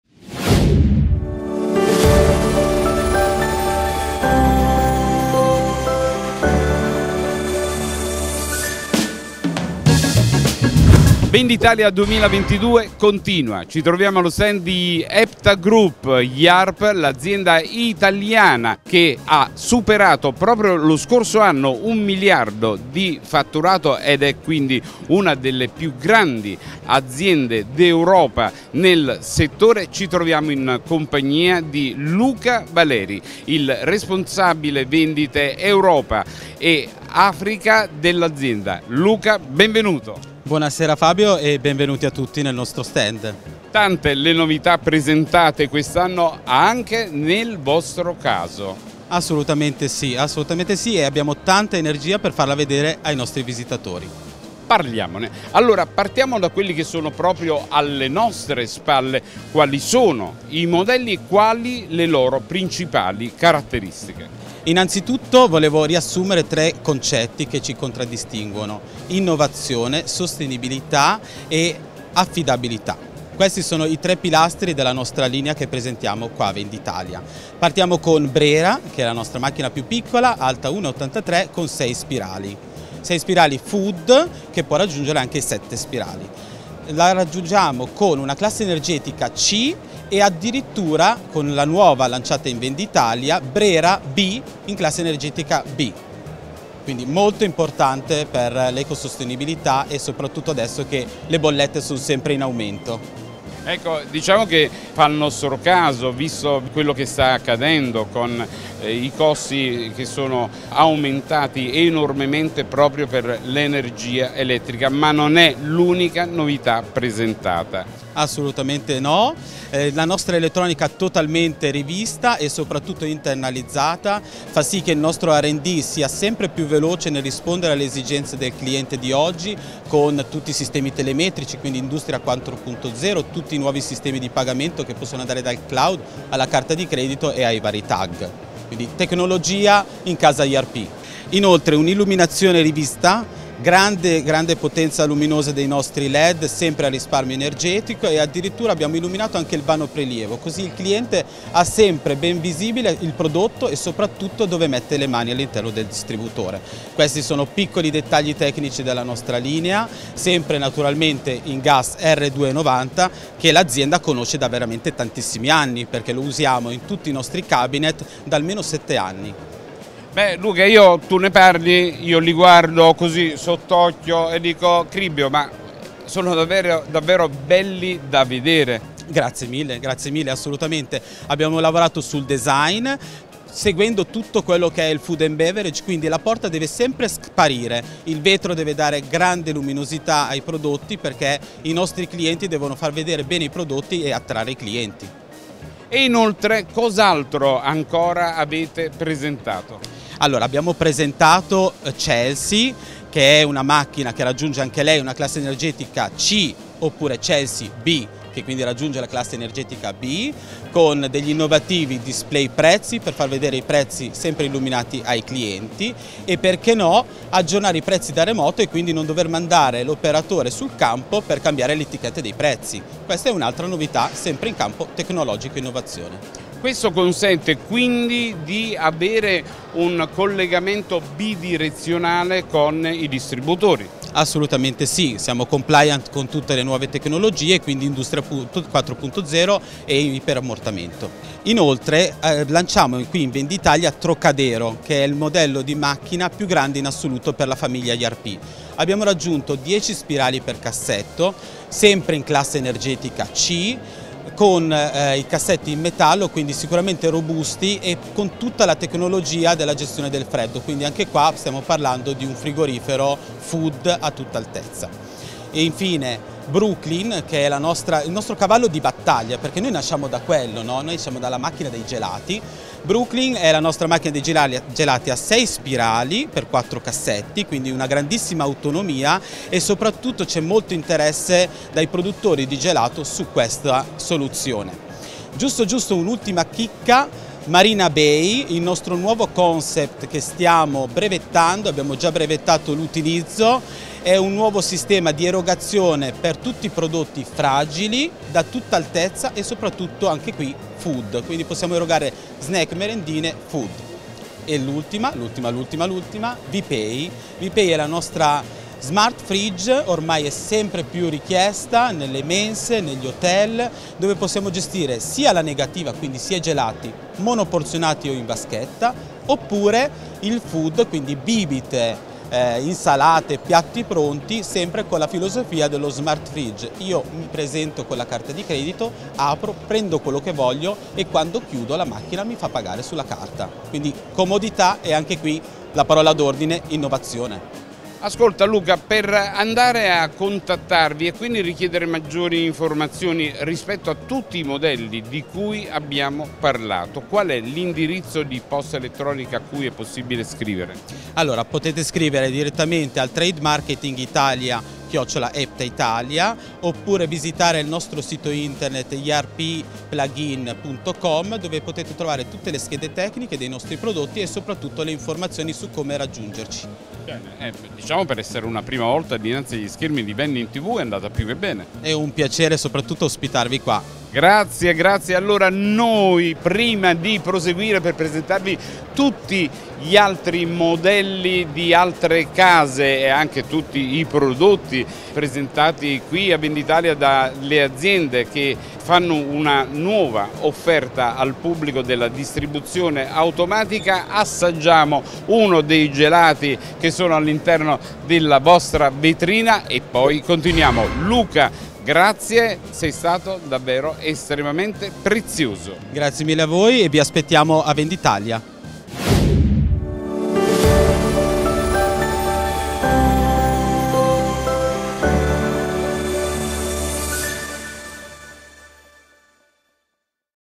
VENDITALIA 2022 - Intervista con Luca Valeri, Responsabile Vendite EMEA di IARP EPTA Group SpA
Venditalia 2022: Fabio Russo intervista per VendingTv Luca Valeri, Responsabile Vendite EMEA di IARP - EPTA Group SpA